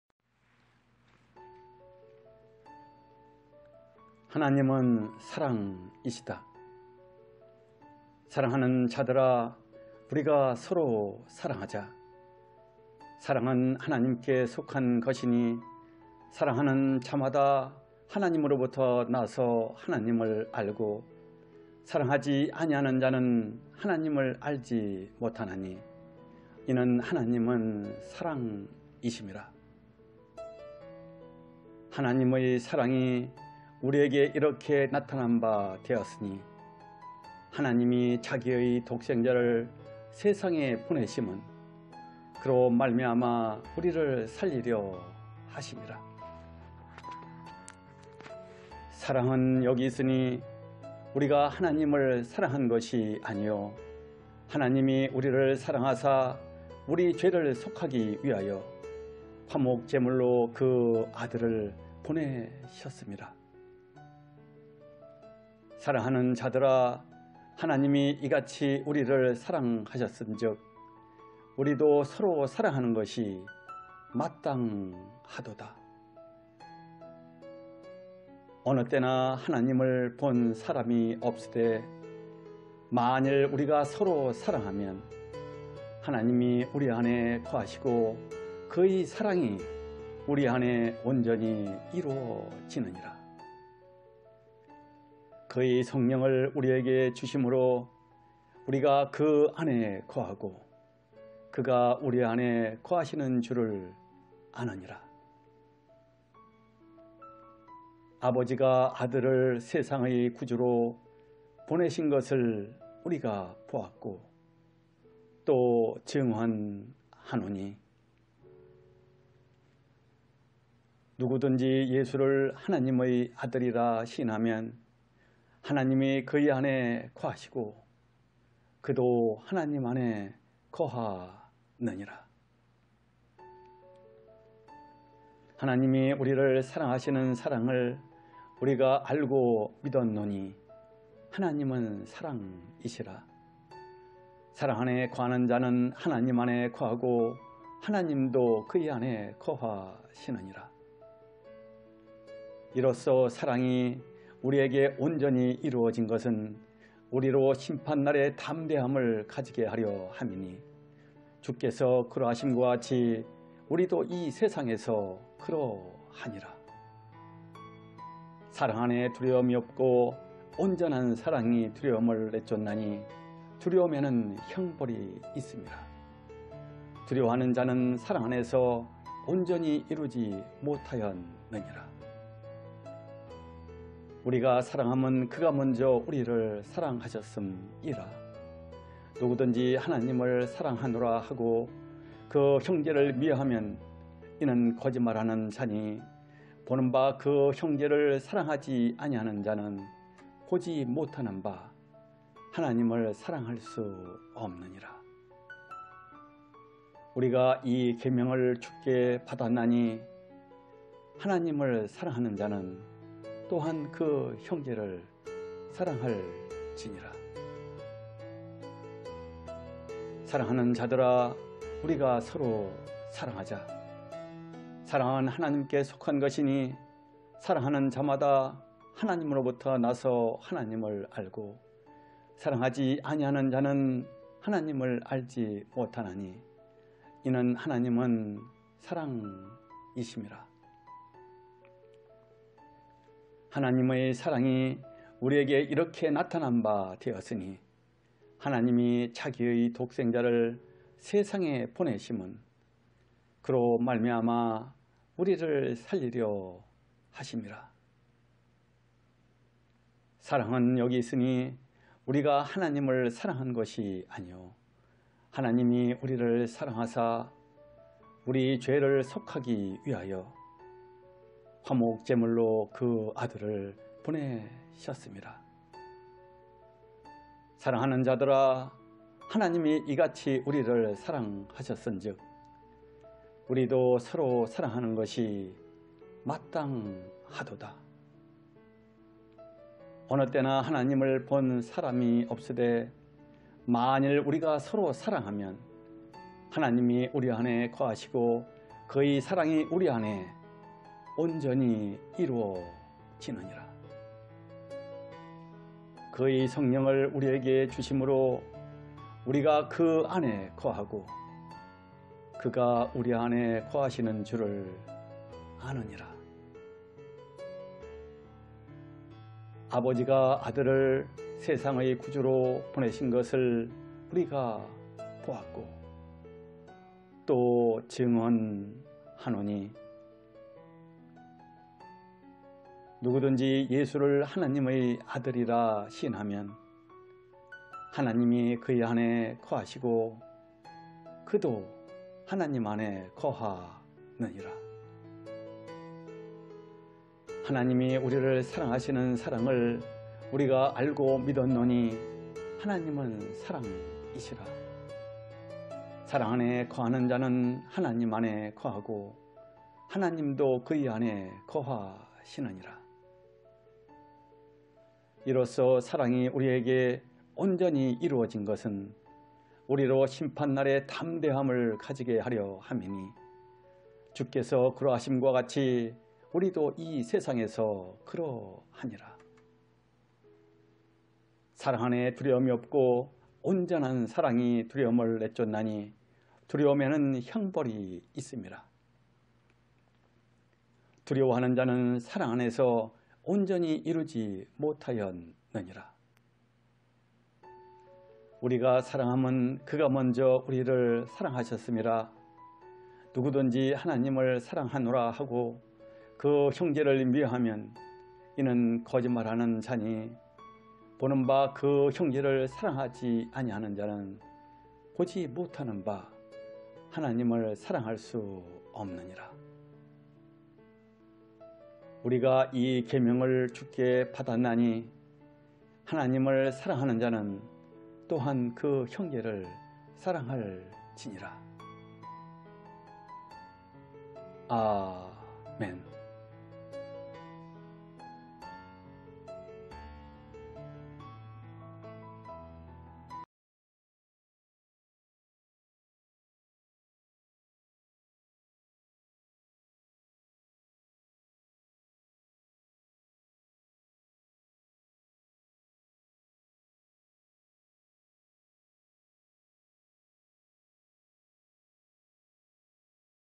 [매일말씀] 말씀낭송 - 하나님은 사랑이시다 (요일 4:7-21)
오늘 매일말씀은 하나님의 보배로운 말씀 낭송(2회 반복),
하나님은 사랑이시다(요일 4:7-21)입니다.

♡ ‘구독’ 부탁드립니다. ‘구독’ 하시면 새로 올라오는
   은혜로운 매일성경 영상을 시청하실 수 있습니다.^^